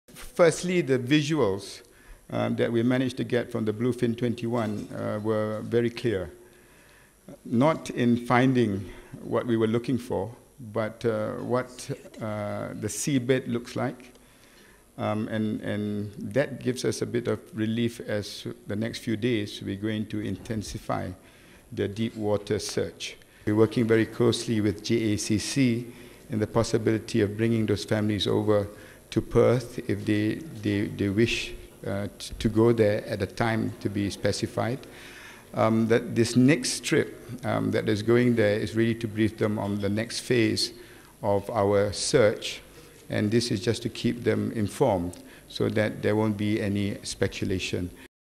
Malaysia's defense minister said on Thursday that images retrieved from the underwater vehicle searc
(17 Apr 2014) FOR CLEAN VERSION SEE STORY NUMBER: 940529

Malaysia's defense minister said on Thursday that images retrieved from the underwater vehicle searching for MH370 were "very clear" but had so far returned no evidence of the missing flight.
Hishamuddin Hussein, who is also the acting transport minister, told media at a news conference in Kuala Lumpur that the clarity of the Bluefin 21's images "gives us a bit of relief" as deep water searches are planned to continue over the coming days.
The unmanned sub's first two missions were cut short by technical problems and deep water, but it finally managed to complete a full 16-hour scan of the silt-covered seabed far off Australia's west coast on Thursday.
Hussein also said they were working with the Joint Agency Coordination Centre (JACC) on bringing relatives of missing passengers from Beijing to Perth.
He said a Malaysian delegation will be sent to Beijing to brief the families on the next phase of the search to ensure "there won't be any speculation".
Despite weeks of searching, no debris related to the jet has yet been found.